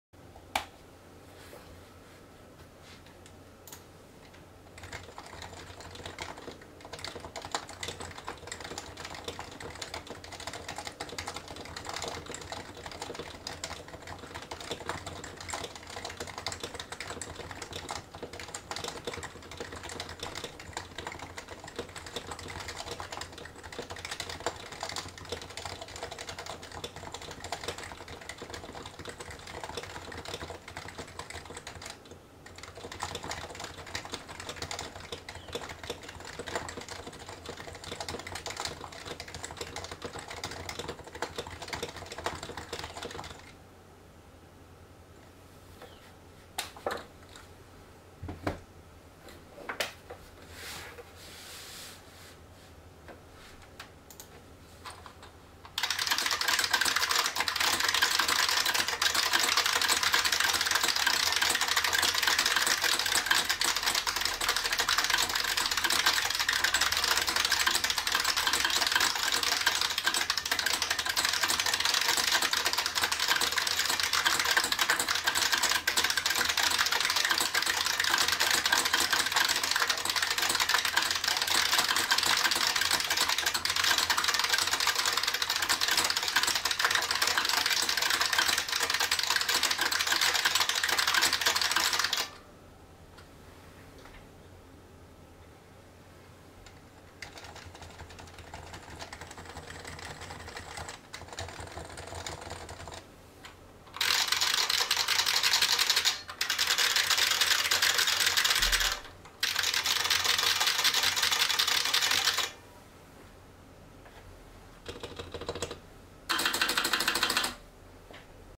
Silenced Topre vs Buckling Spring
The comparison nobody asked for!

HHKB Hybrid Type-S stock w vibration mat vs IBM Model F AT